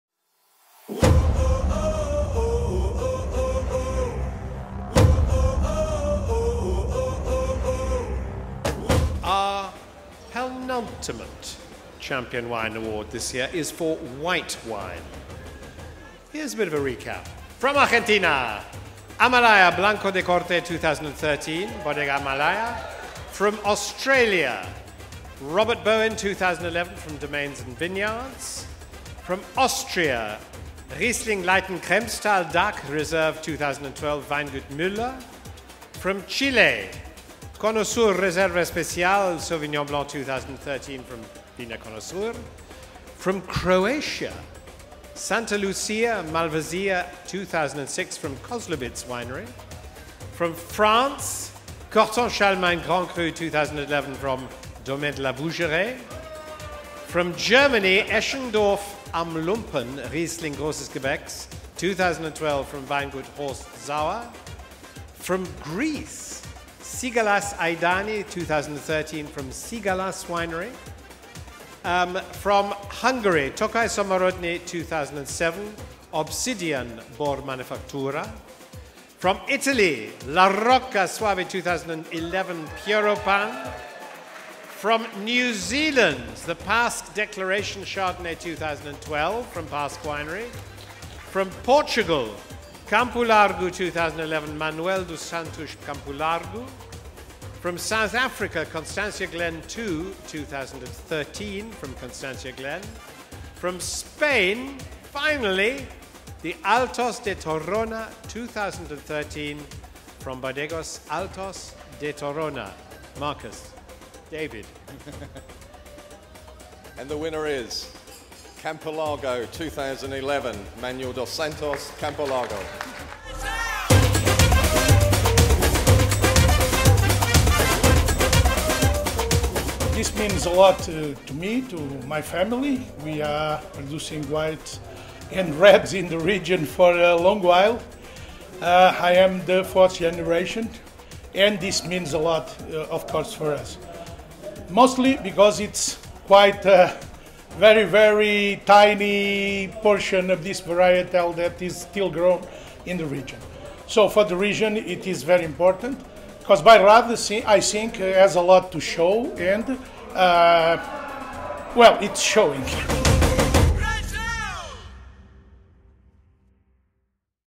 IWC2014 Champion White Wine – Campolargo 2011, Manuel dos Santos Campolargo
Peppery, flint and cashew richness seduces on the nose. Complex and serious with restrained oak spice balancing lemon preserve. Fleshy, full, rounded and creamy. Pure focused acidity which provides a youthful freshness to the end.